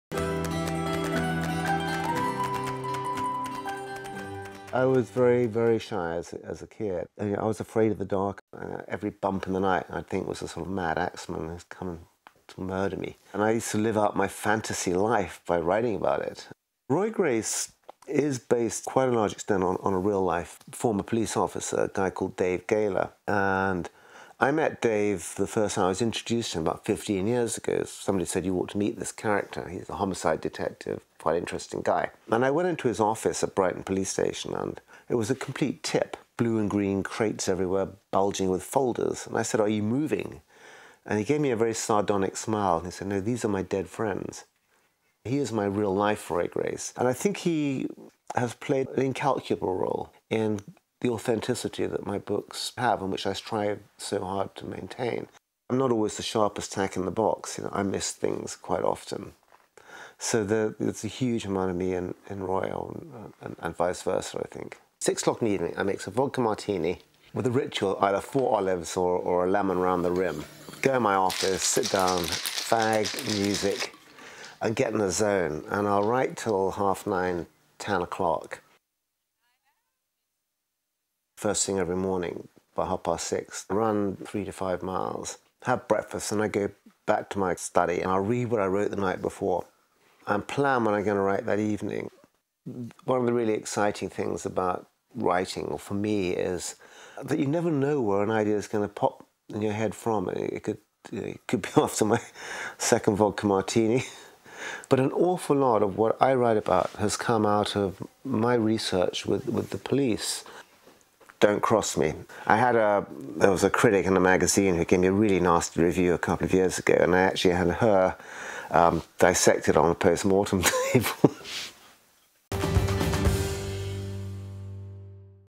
The Writer's Room: Peter James
Come and see where Peter James pens his internationally bestselling Roy Grace novels, learn about his enviable writing routine and explore his collection of police memorabilia!